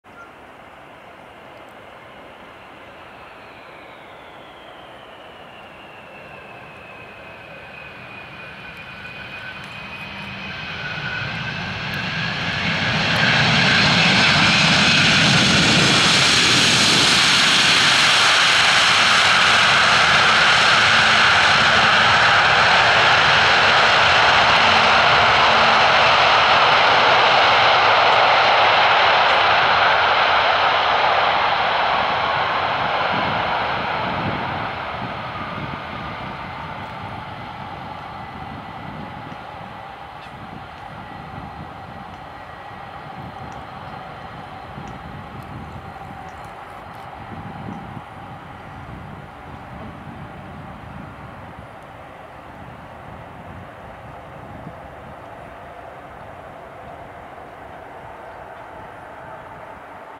FULL THRUST REVERSER on this MD-11 sky lease cargo N951AR @ AMS Schiphol
FULL THRUST REVERSER on this MD-11 sky lease cargo N951AR @ AMS Schiphol after landing on the polderbaan on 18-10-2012 at 1700 hour

Aircraft Type                           McDonnell Douglas MD-11
Date of                                      Manufactur 04-1991
Age                                           21.6 Years
Airframe Status                   Active